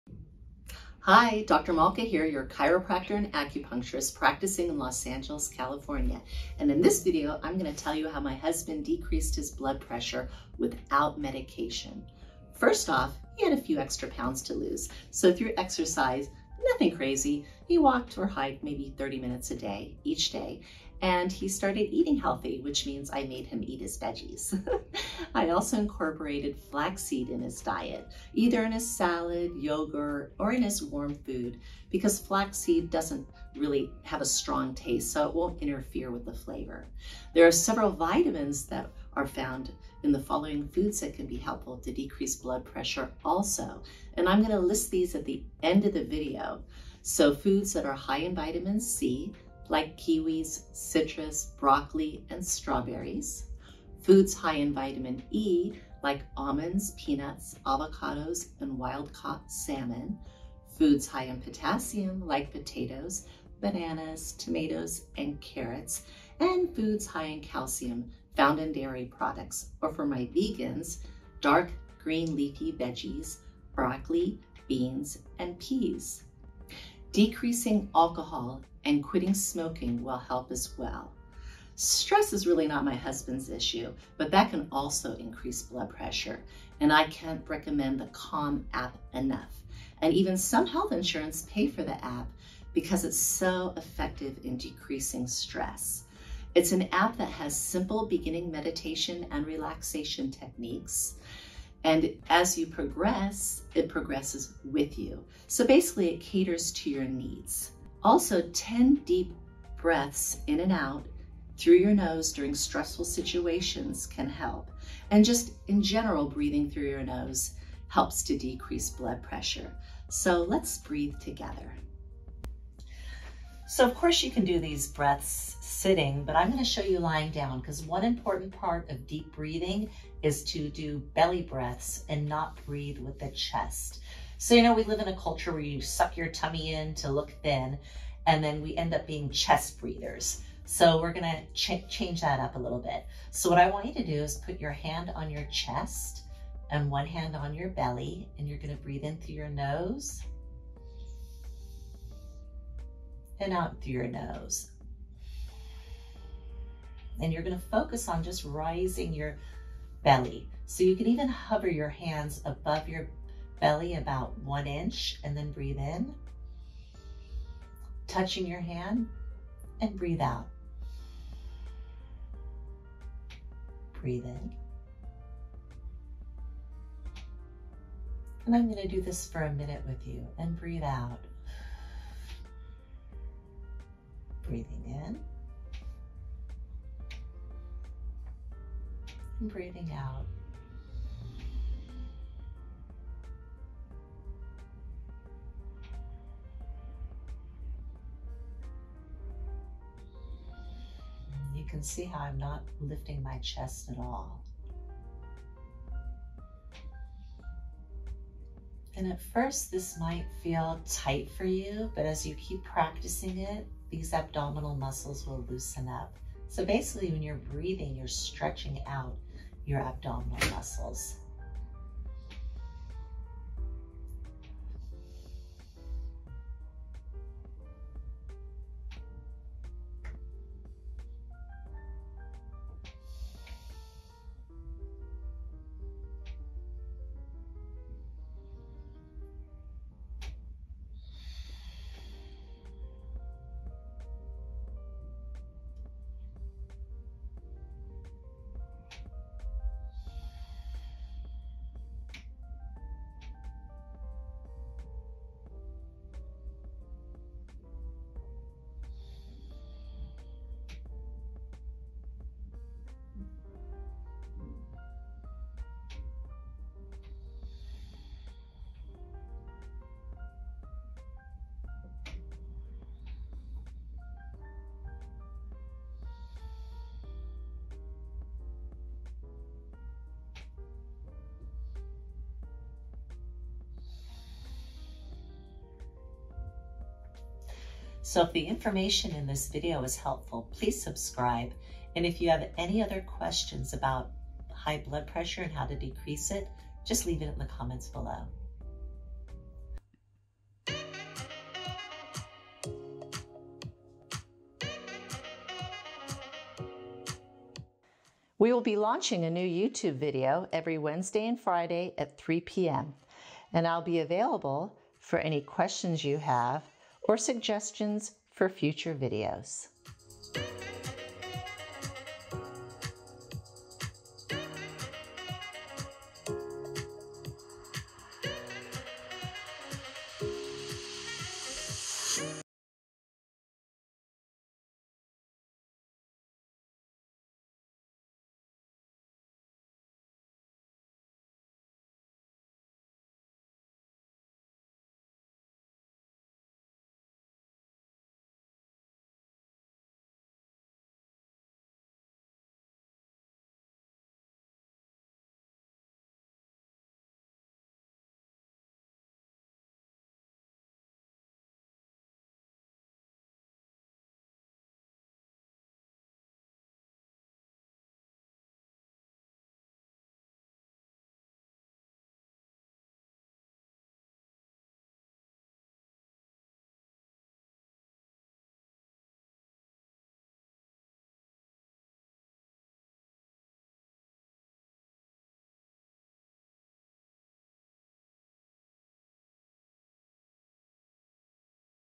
REDUCE YOUR BLOOD PRESSURE NATURALLY!!!
In this video I will discus how you can reduce your blood pressure by making a few changes and adding essential foods to your diet